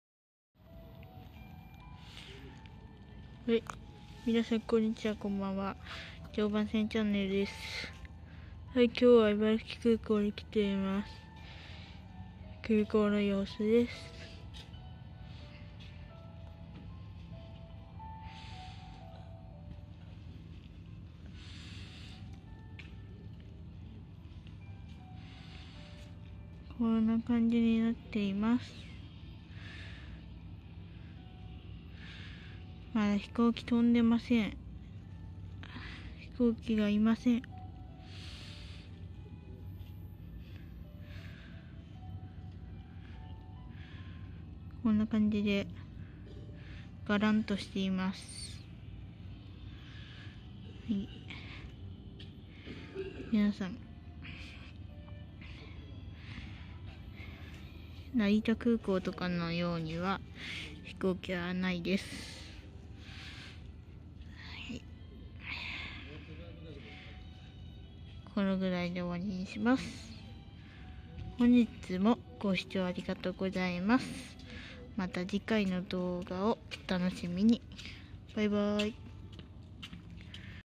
茨城空港
茨城空港に行って来ました！
自衛隊の航空機は飛んでましたが、旅客機は、飛んでませんでした。
送迎デッキでの様子です！